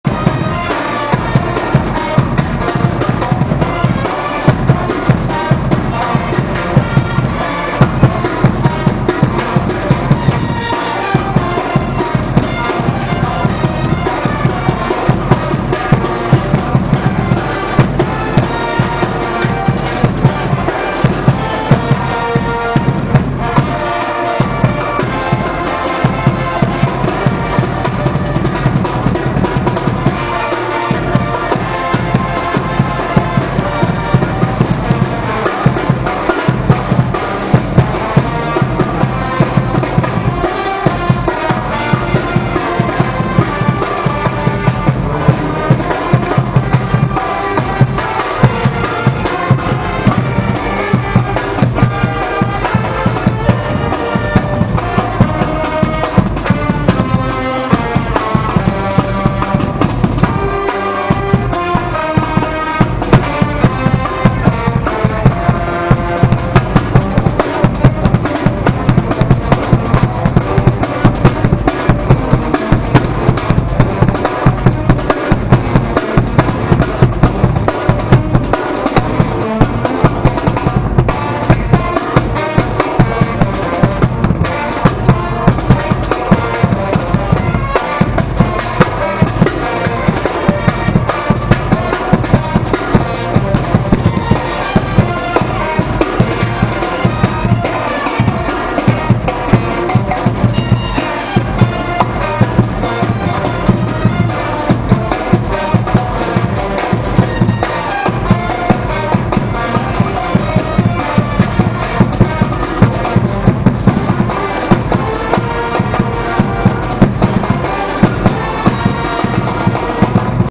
Yeah Standford
I was at this hotel in San Jose, CA and low and behold... cheerleaders! God I love the USA.

They sure did cheer up the place.